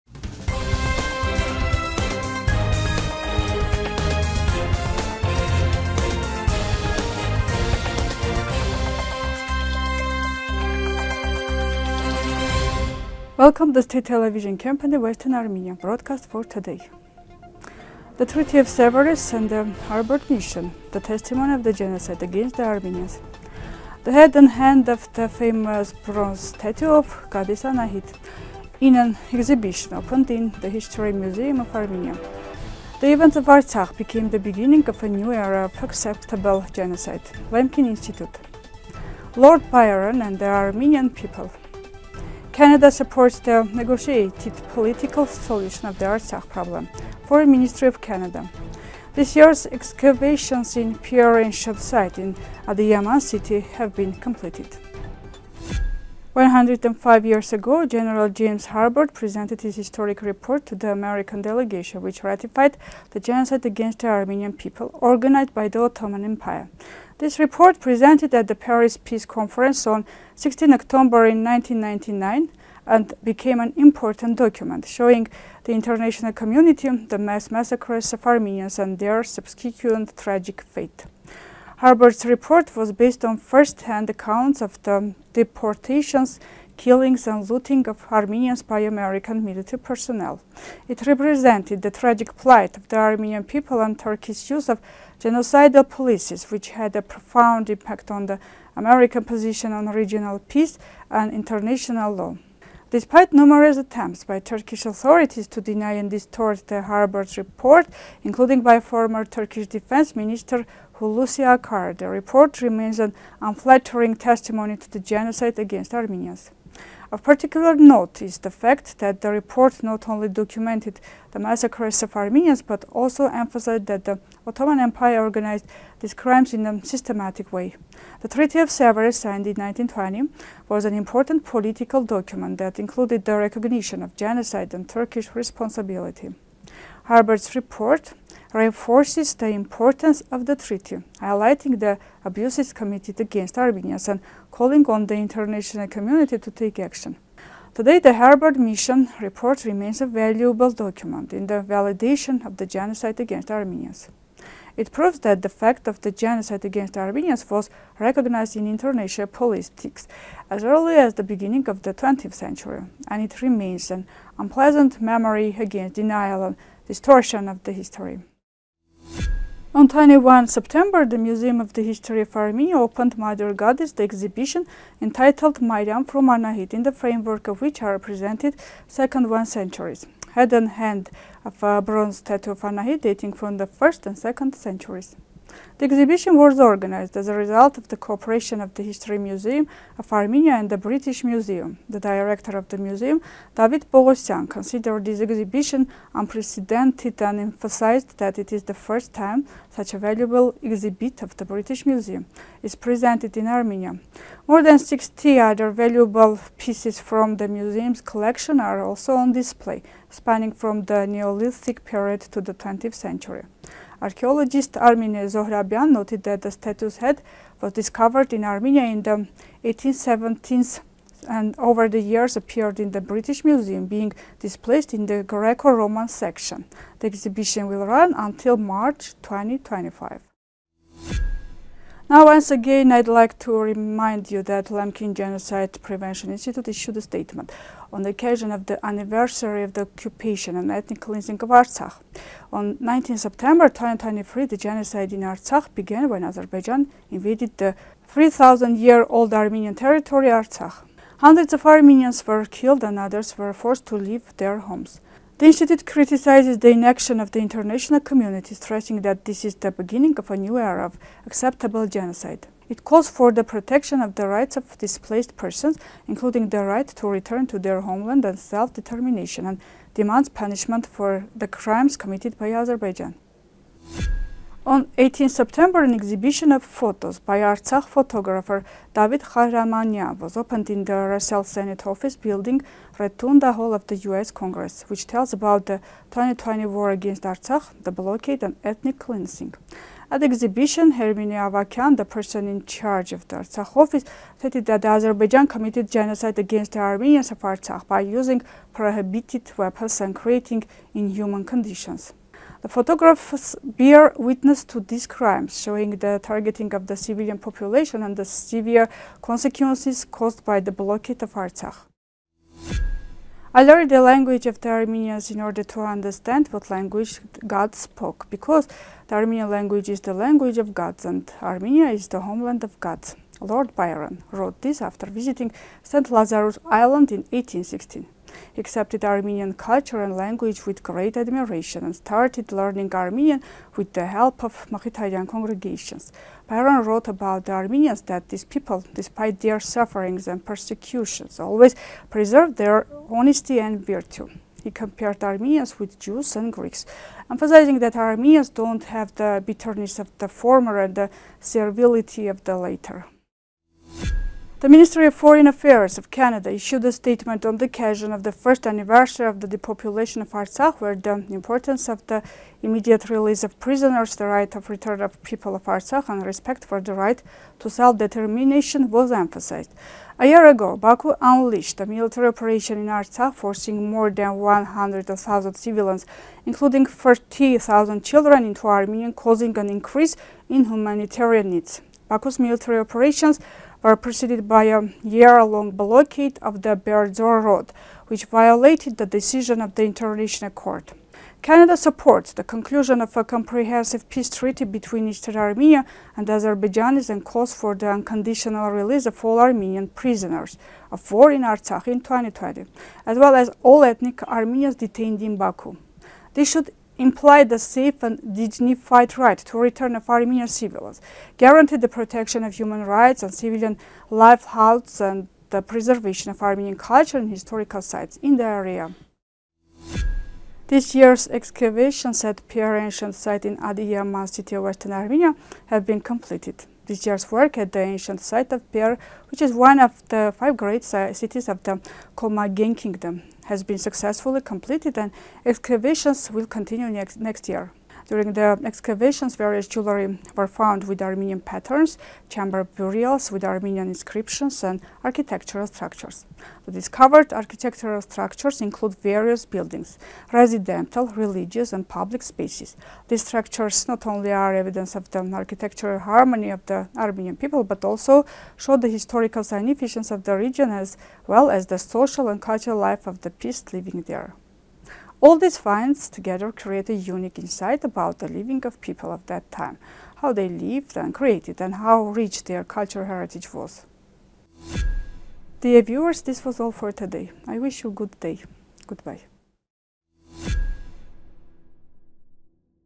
The Events of Artsakh Became the Beginning of a New Era of "Acceptable Genocide". Lemkin Institute
1. The Treaty of Sèvres and the Harbord Mission, the Testimony of the Genocide Against the Armenians

2. The Head and Hand of the Famous Bronze Statue of  Goddess Anahit, in An Exhibition Opened in the History Museum of Armenia

3. The Events of Artsakh Became the Beginning of a New Era of "Acceptable Genocide". Lemkin Institute

4. The Photos of Davit Ghahramanyan, a Photographer Displaced from Artsakh, Document the Atrocities in Azerbaijan. "ration"

5. Lord Byron and the Armenian People

6. Canada Supports the Negotiated Political Solution of the Artsakh Problem. Foreign Ministry of Canada

7. This Year's Excavations in Perre Ancient Site in Adiyaman City Have Been
Completed